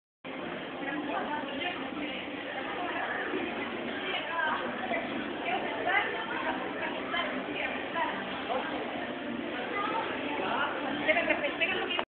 Anime Festival Chile 15 dic 2007 (19 de 21) Cosplay 17
Anime Festival Chile 15 dic 2007 (19 de 21) Cosplay 17. Haruhi, Mikuru y Yuki (TMoHS).

Visita mi flog y mi tienda en deremate-Chile. Gracias!